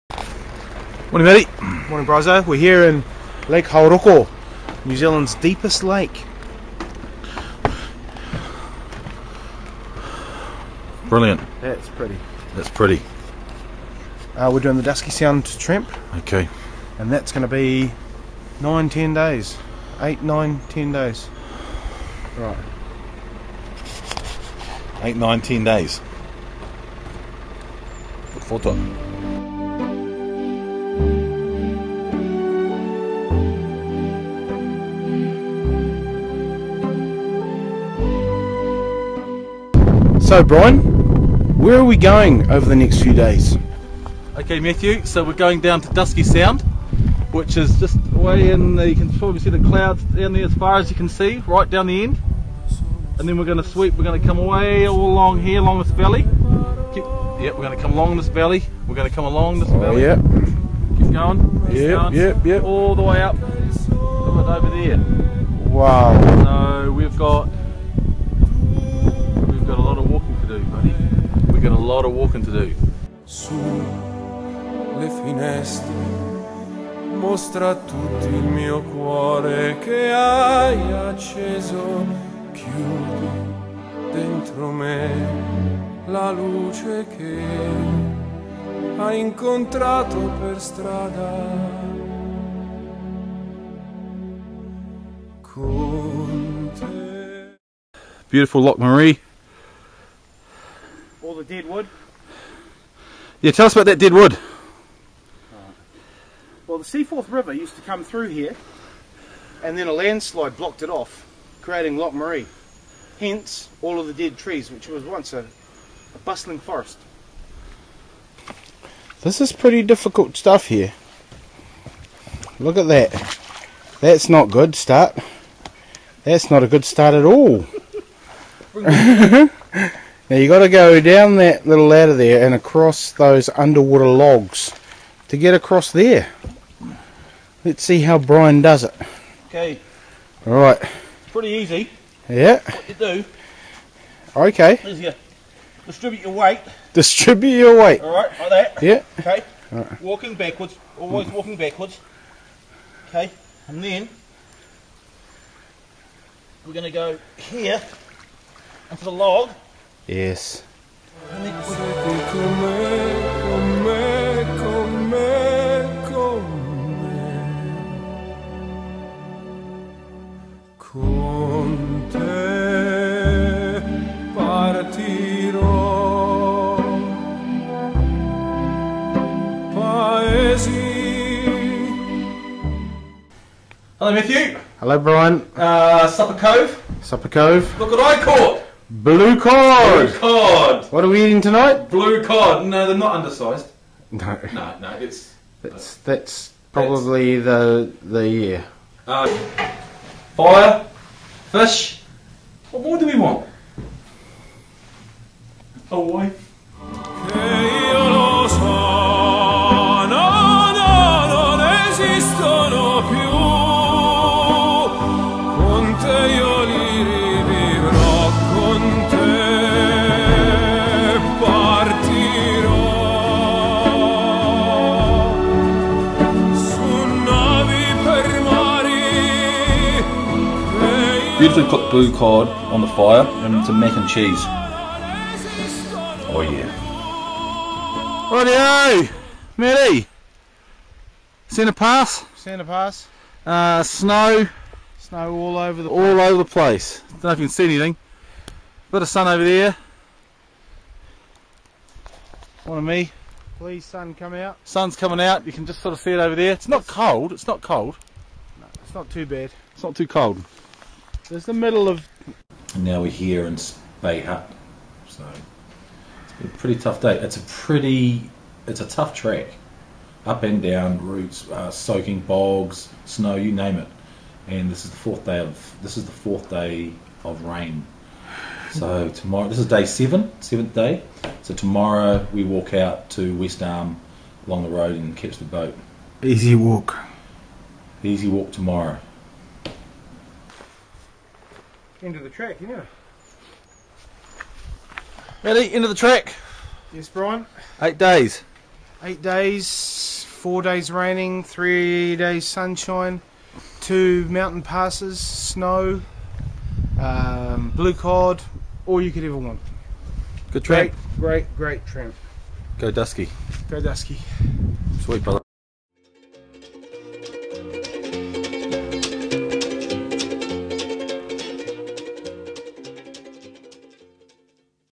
Dusky Track Fiordland Samoan Trampers
The boys kick off 2012 with an eight day tramp through the heart of Fiordland national Park. Challenging, scenic, rewarding. Great fishing at Dusky sound. Had fun battling the elements.